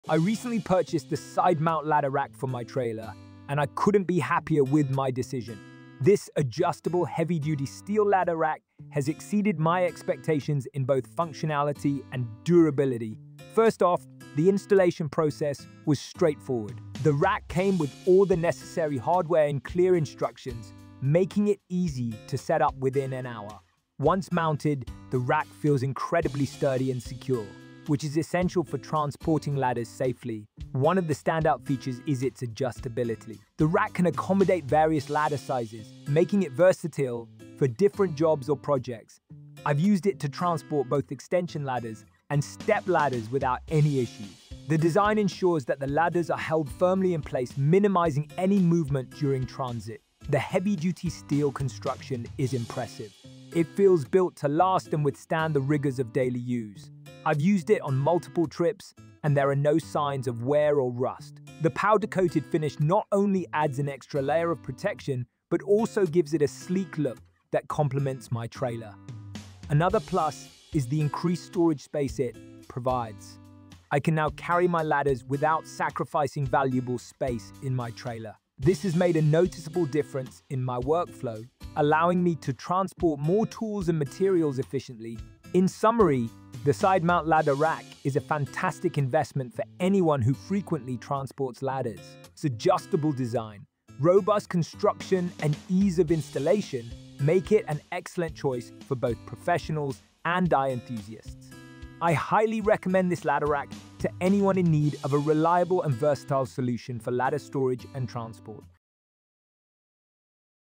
Side Mount Ladder Rack For Trailer Adjustable Heavy Duty Steel Ladder Rack Review
I recently purchased the Side Mount Ladder Rack for my trailer, and I couldn't be happier with my decision. This adjustable heavy-duty steel ladder rack has exceeded my expectations in both functionality and durability.

First off, the installation process was straightforward. The rack came with all the necessary hardware and clear instructions, making it easy to set up within an hour. Once mounted, the rack feels incredibly sturdy and secure, which is essential for transporting ladders safely.

One of the standout features is its adjustability. The rack can accommodate various ladder sizes, making it versatile for different jobs or projects. I’ve used it to transport both extension ladders and step ladders without any issues. The design ensures that the ladders are held firmly in place, minimizing any movement during transit.

The heavy-duty steel construction is impressive; it feels built to last and withstand the rigors of daily use. I’ve used it on multiple trips, and there are no signs of wear or rust. The powder-coated finish not only adds an extra layer of protection but also gives it a sleek look that complements my trailer.

Another plus is the increased storage space it provides. I can now carry my ladders without sacrificing valuable space in my trailer. This has made a noticeable difference in my workflow, allowing me to transport more tools and materials efficiently.

In summary, the Side Mount Ladder Rack is a fantastic investment for anyone who frequently transports ladders. Its adjustable design, robust construction, and ease of installation make it an excellent choice for both professionals and DIY enthusiasts. I highly recommend this ladder rack to anyone in need of a reliable and versatile solution for ladder storage and transport.